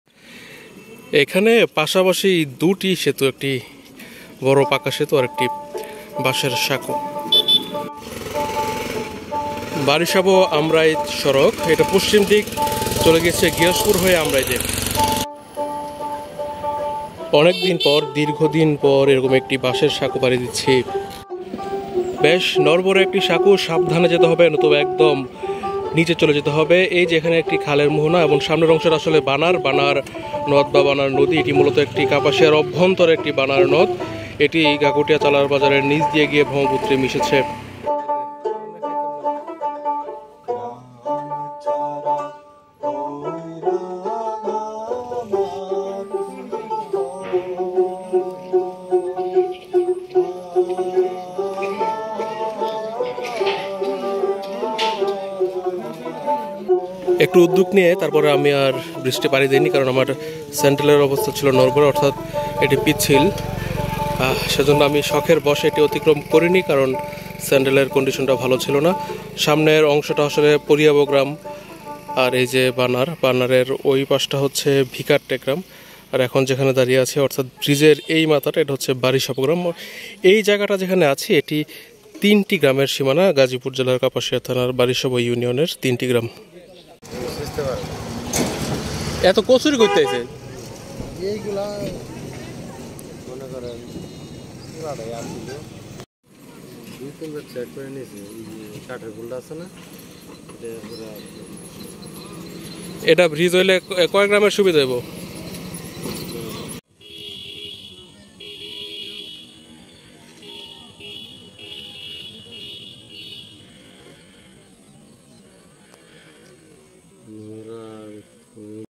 বারিষাব ইউনিয়নের তিনটি গ্রামের সীমানা এখানে।
গাজীপুর জেলার কাপাসিয়া থানার বারিষাব ইউনিয়নের তিনটি গ্রামের সীমানা এখানে।বারিষাব,পরিয়াব, এবং ভিকারটেক।